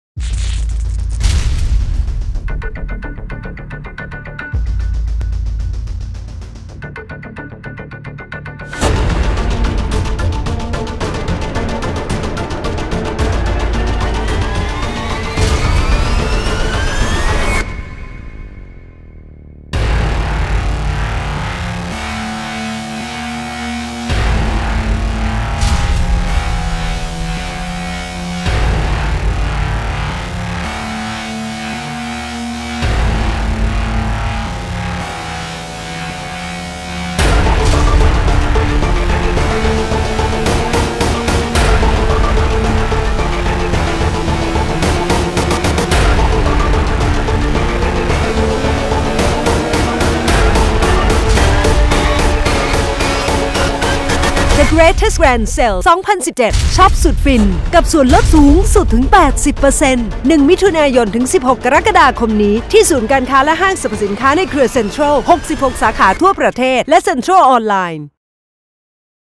The Greatest Grand Sale 2017
How far would you go for sale?
เตรียมตัวมาช้อปสุดฟินพร้อมกัน
1 มิ.ย -16 ก.ค นี้ กับงาน SALE ที่ดีที่สุดแห่งปี ลดสูงสุดถึง 80% จาก 10,000 แบรนด์ TheGreatestGrandSale2017 ช้อปสุดฟิน ที่ศูนย์การค้าเซ็นทรัล และห้างสรรพสินค้าในเครือเซ็นทรัล 66 สาขาทั่วประเทศ และเซ็นทรัลออนไลน์